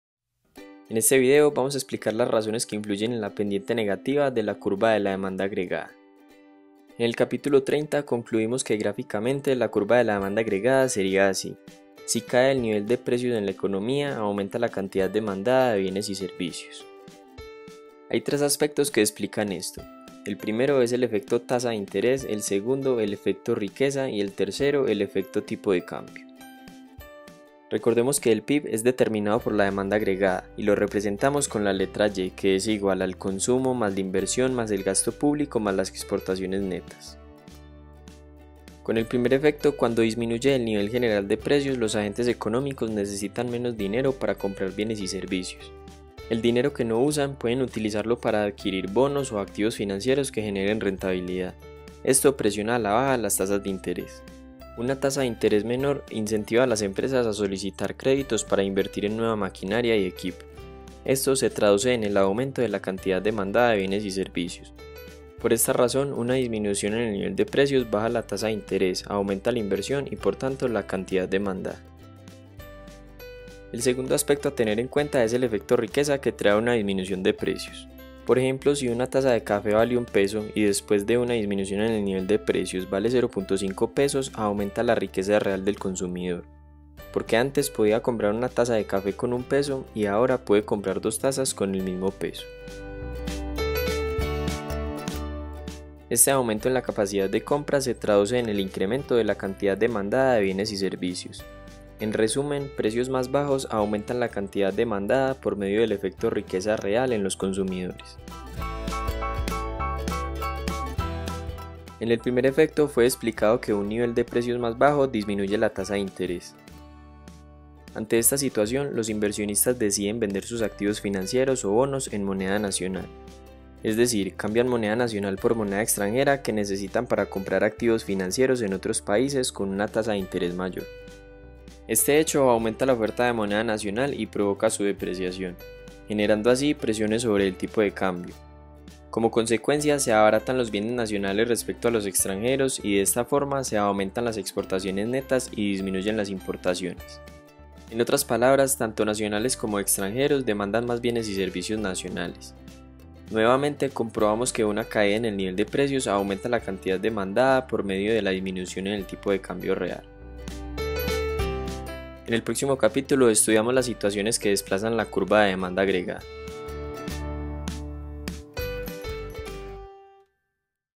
Curva de demanda agregada, razones de su pendiente negativa | Cap. 31 - Macroeconomía
En este capítulo vamos a explicar las razones que influyen en la pendiente negativa de la curva de demanda agregada.

Fuentes:

- De Gregorio, J (2012). Macroeconomía. Teoría y políticas. Santiago: Pearson Educación.